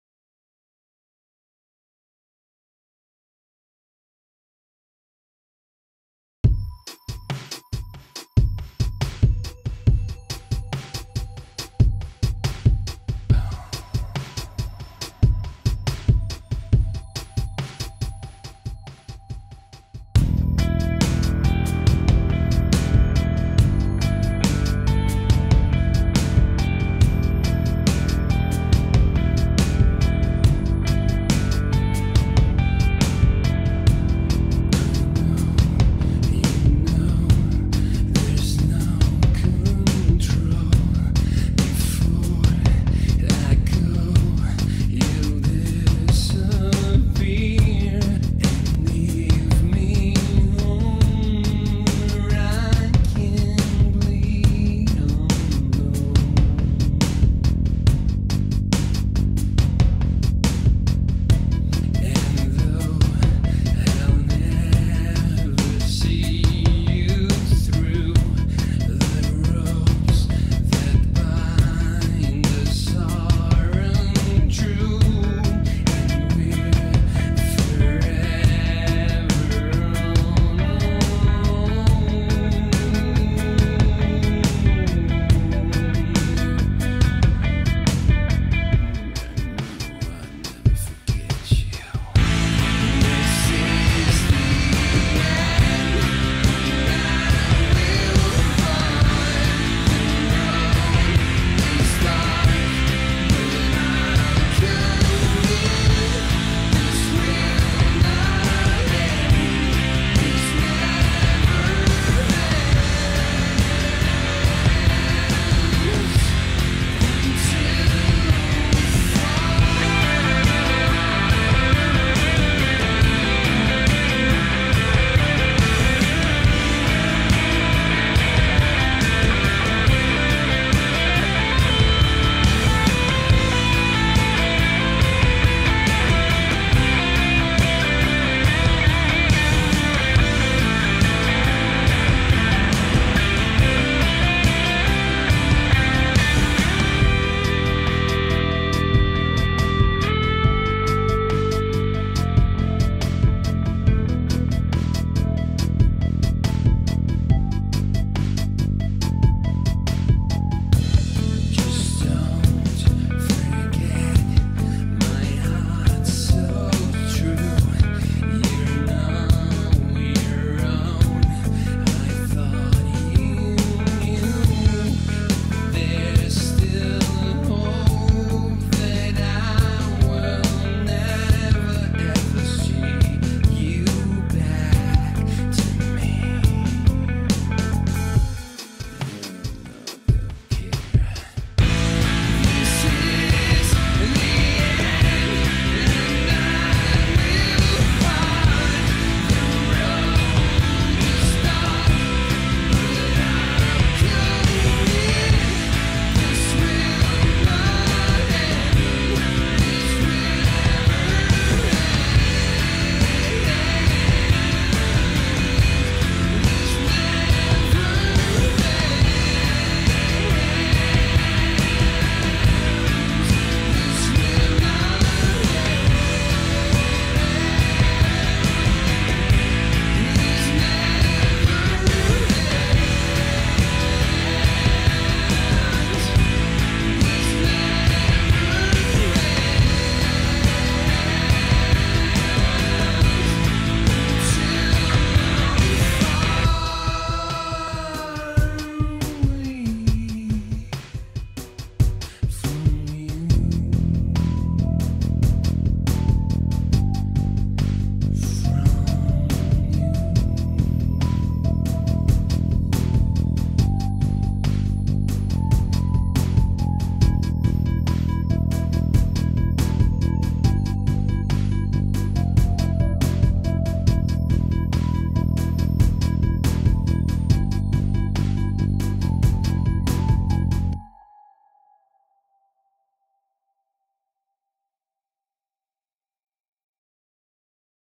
Cinematic Sunset at Bow River - part 2
This is not a music video, it's a drone video set to music. Still mostly experimenting with the best way to use the drone to capture useful shots and make them look reasonably cinematic.